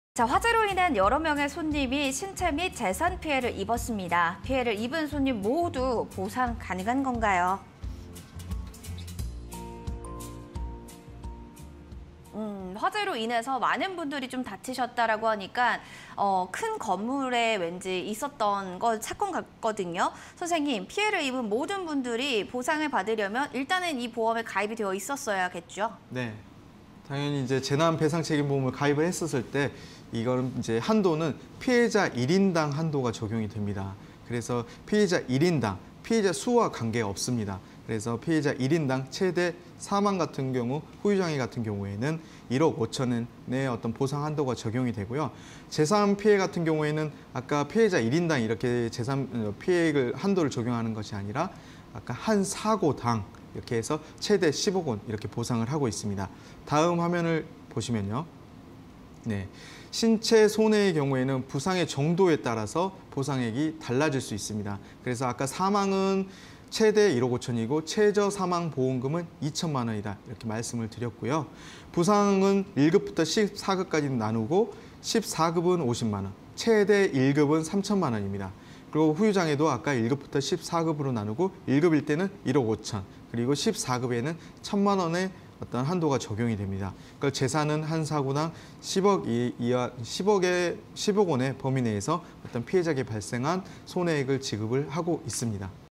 화재로 인해 손님이 신체 및 재산피해를 입을 경우 손님들 모두 보상이 가능할까?_보험 Q&A (20210910)
# 화재로 인해 손님이 신체 및 재산피해를 입을 경우 손님들 모두 보상이 가능할까?